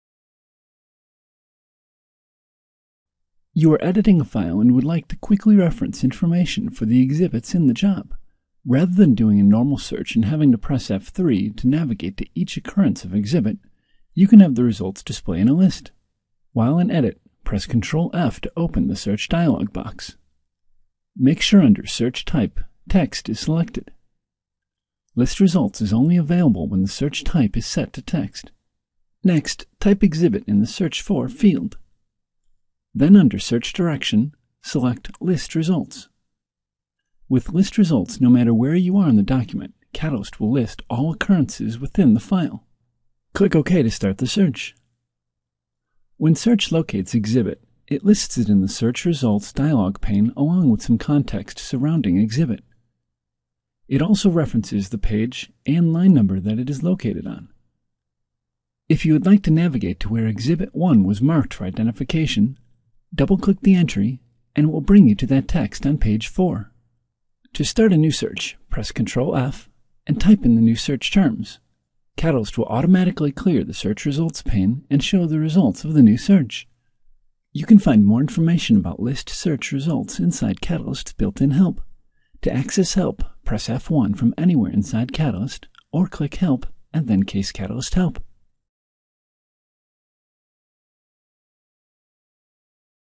CATalyst Training: List Search Results
Video tutorial of how to use CATalyst's List Search Results feature.